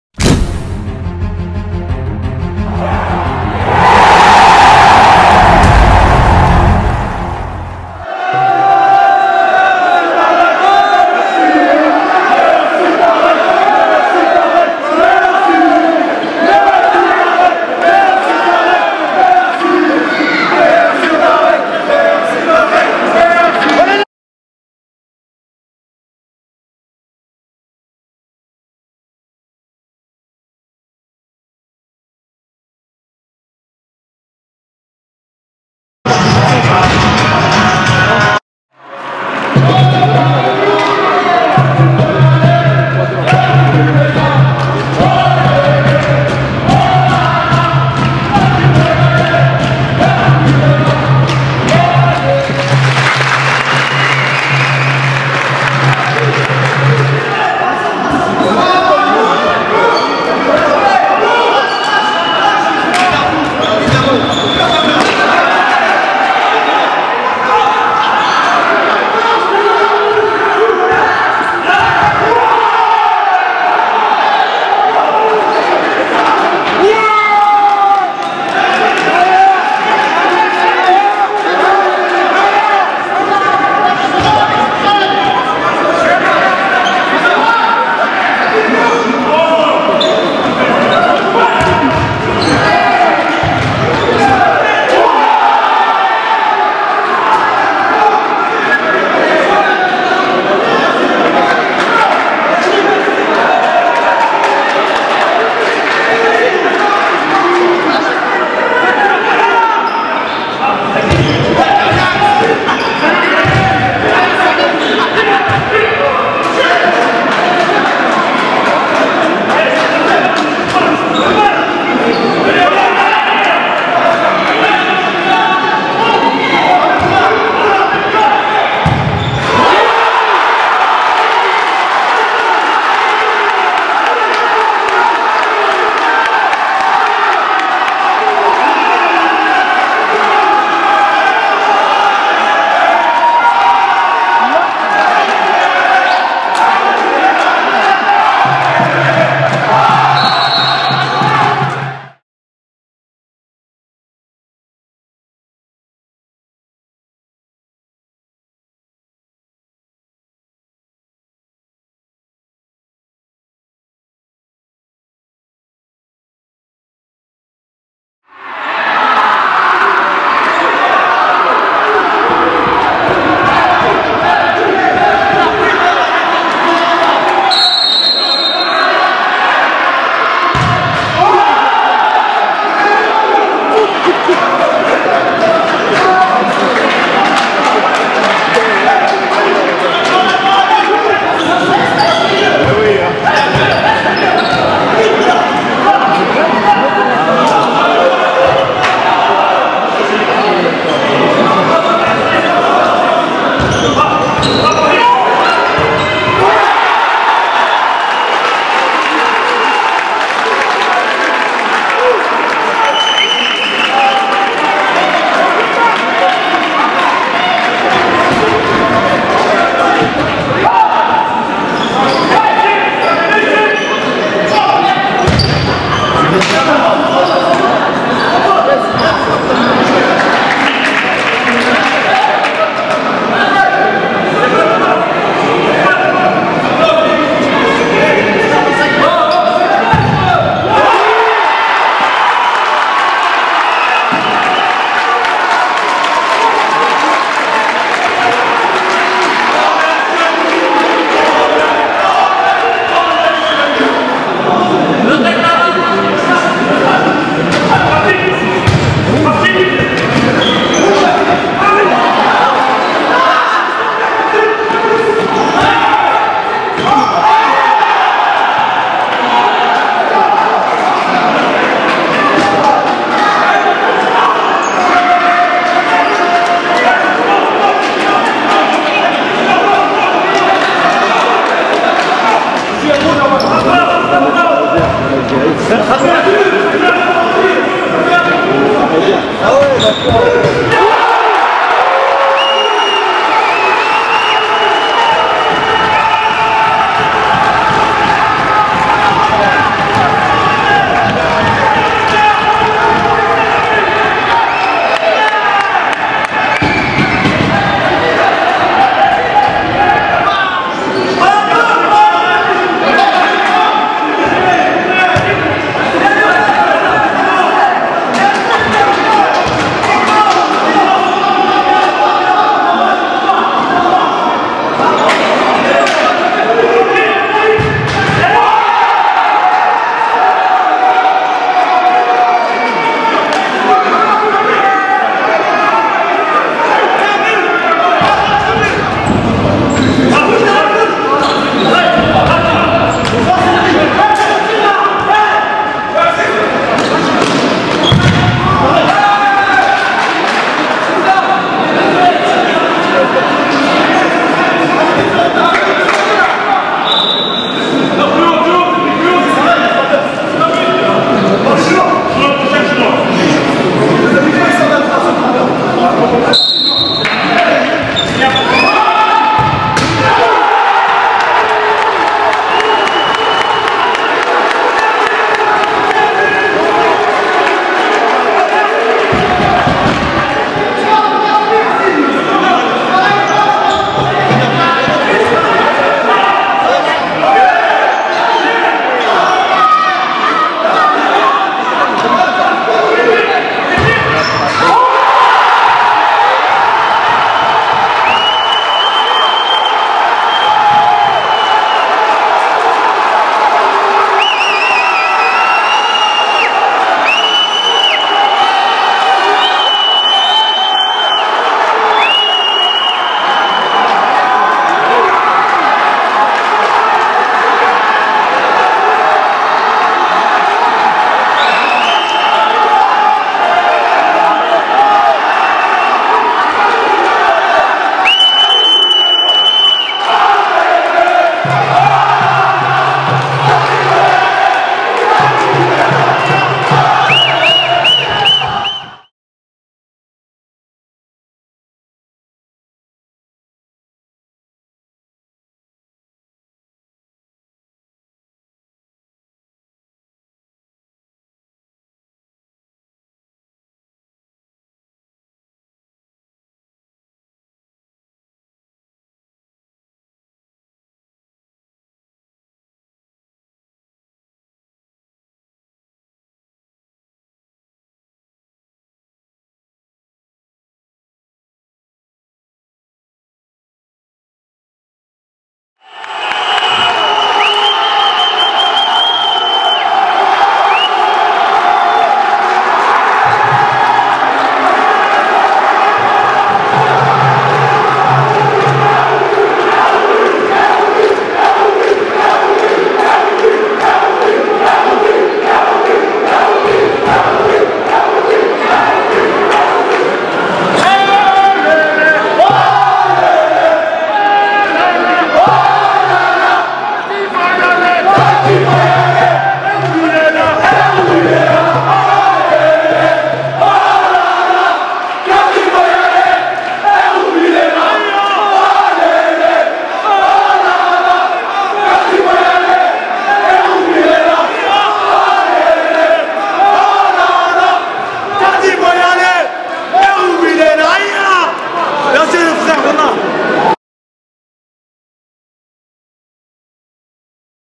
Ligue 2 Futsal : Résumé de la rencontre Herouville Futsal - Paris Acasa Futsal
(Vidéo à voir en HD)

Choc des leaders

Ce Samedi, Herouville Futsal (2ème) recevait dans son antre (Le Gymnase Allende), le leader du Groupe A (Ligue 2 Futsal) Paris Acasa.

Les Herouvillais s'offrent une victoire de prestige en disposant du leader sur le score de (6-5)